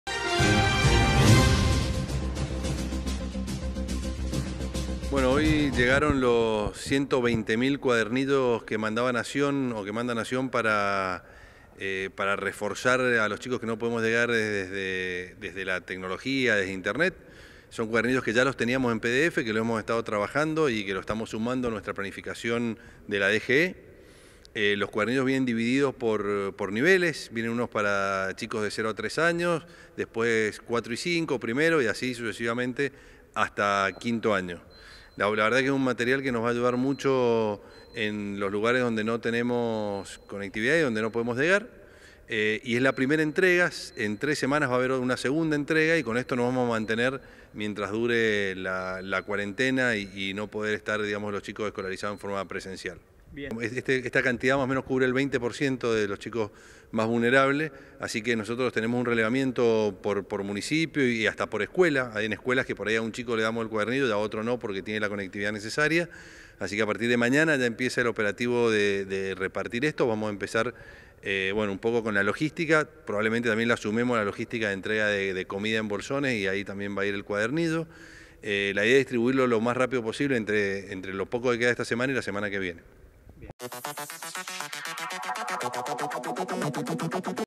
PRIMERO LA EDUCACIÓN
José Thomas, responsable de la Dirección General de Escuelas de la Provincia de Mendoza, anunció la llegada de cuadernillos destinados a los estudiantes que no poseen dispositivos para aprender a través de medios digitales.